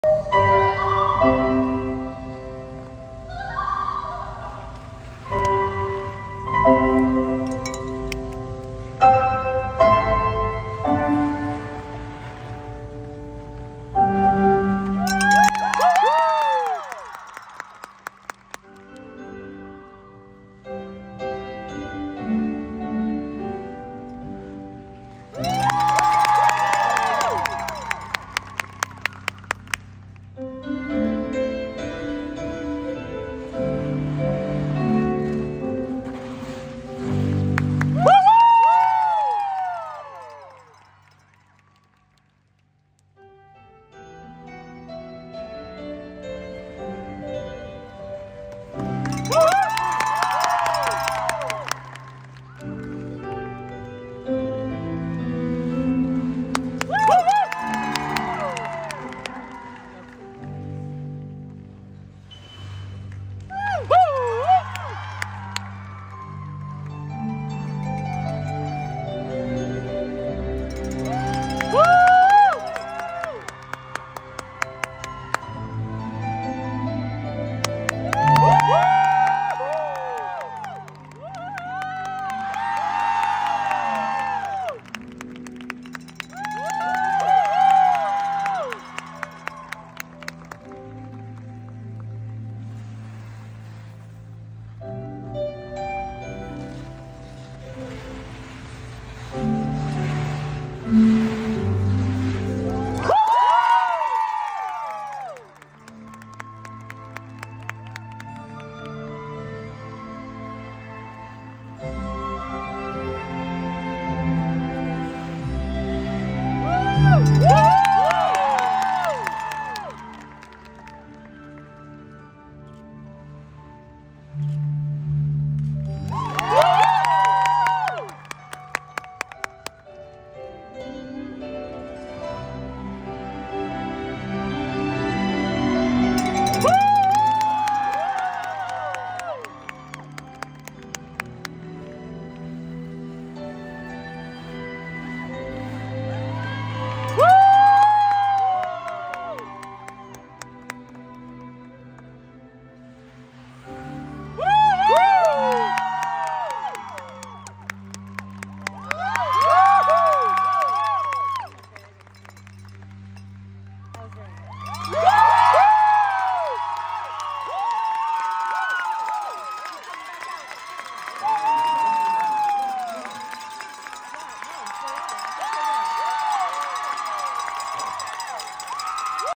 US Synchronized Skating Championship 2024 - Lexettes Short Program
This is the Lexettes short program at the 2024 US Synchronized Skating Championship in Las Vegas.